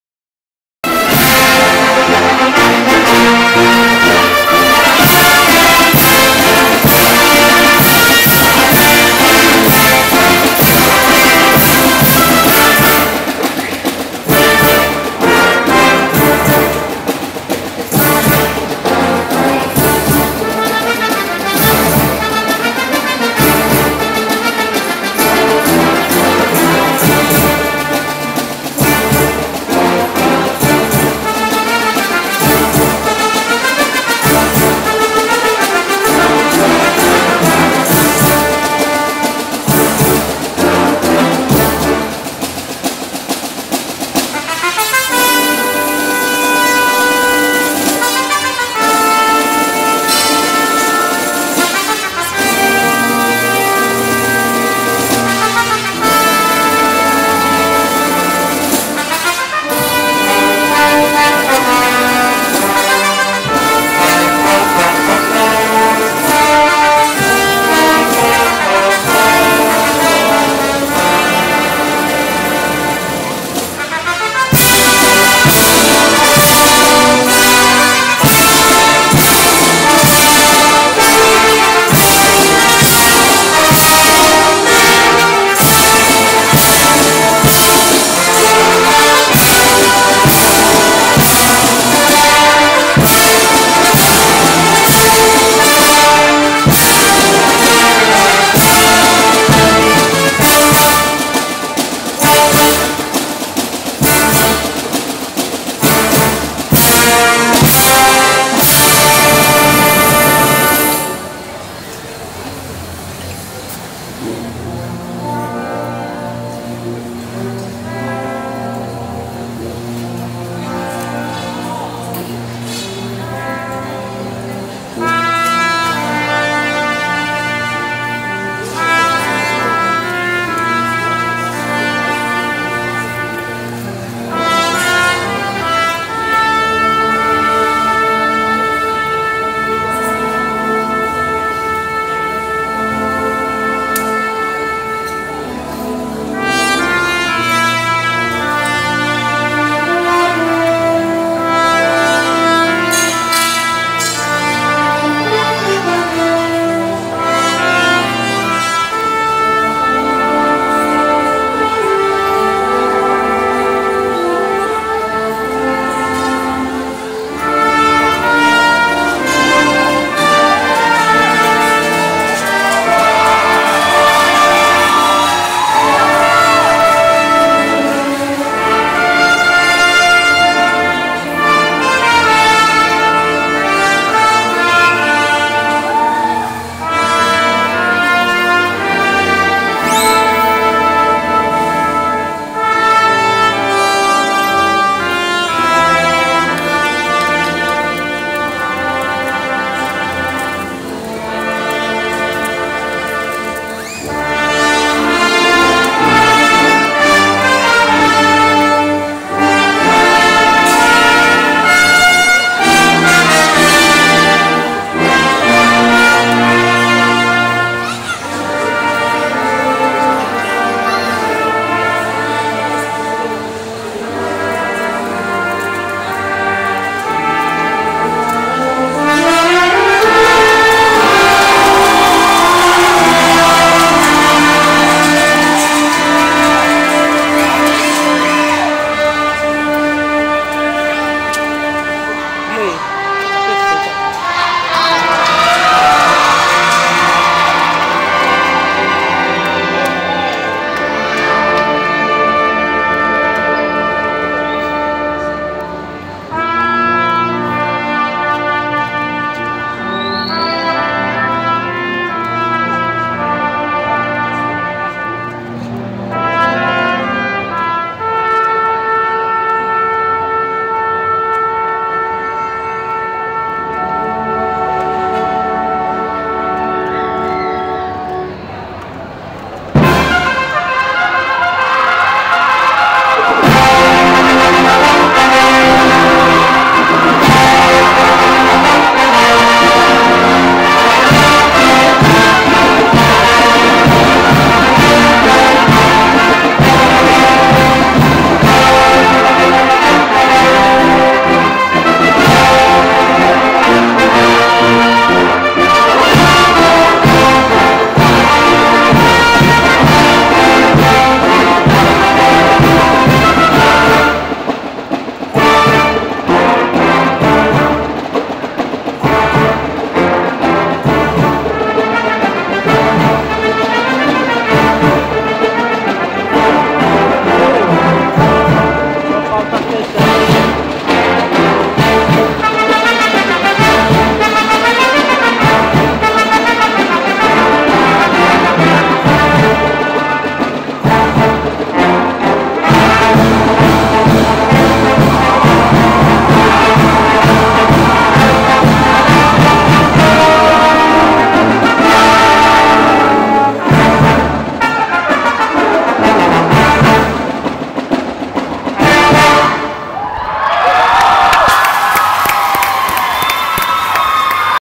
Corporação musical olga benario primeira peça
Bmob da um show em cristinapolis e como premiacao foi desclassificada em concurso da abanfare se